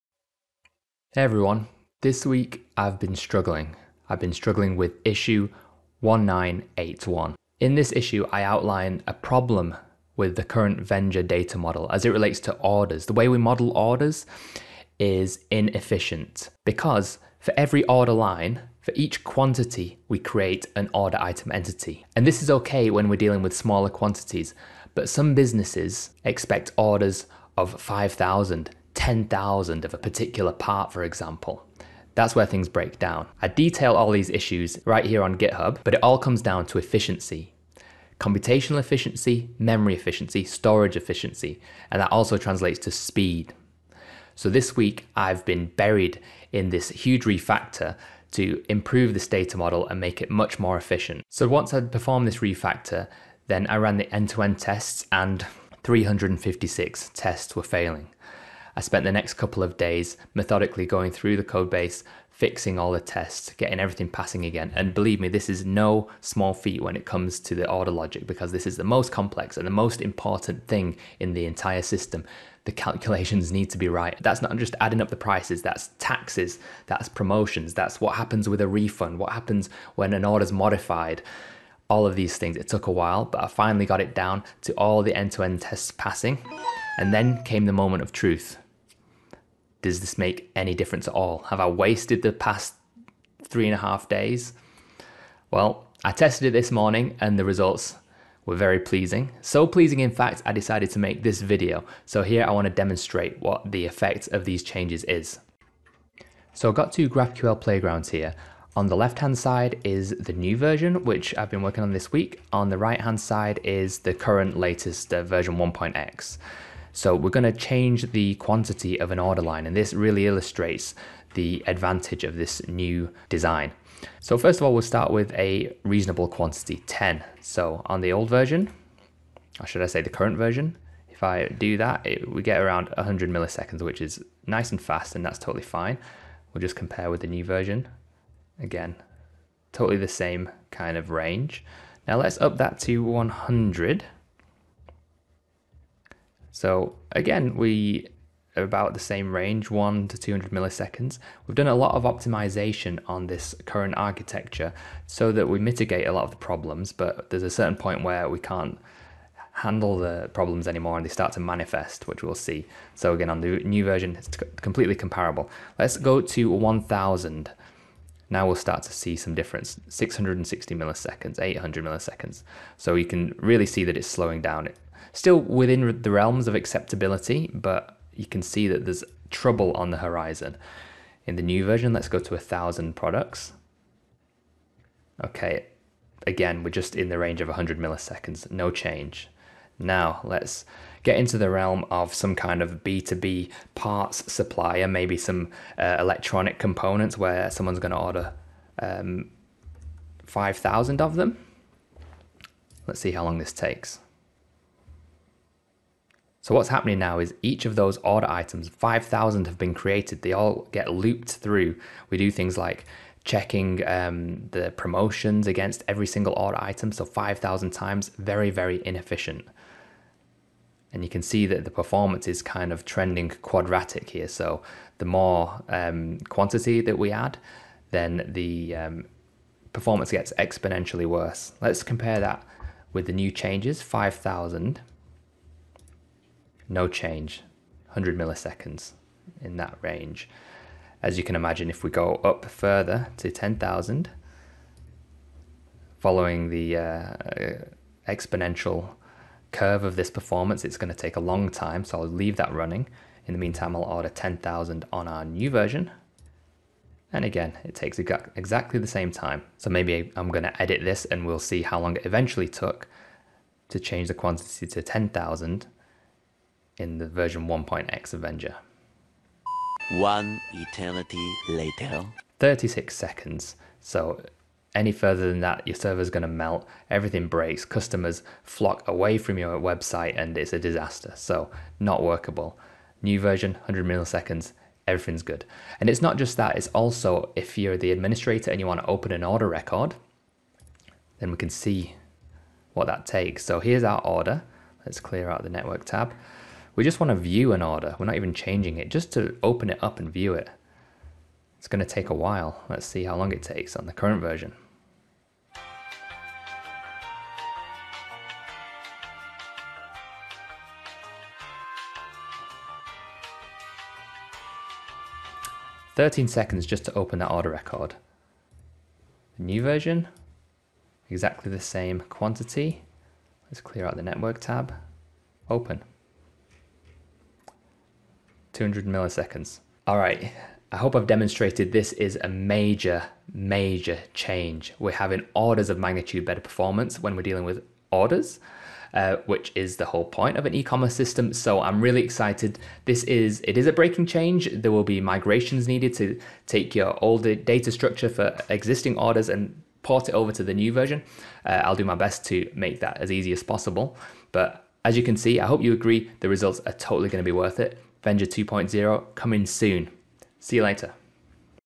Vendure 2: 360x FASTER on large orders!
This is a preview of some major performance work I've been doing in preparation for the release of v2 of Vendure.

Short version: v2 of Vendure will be orders of magnitude more performant when working with very large orders!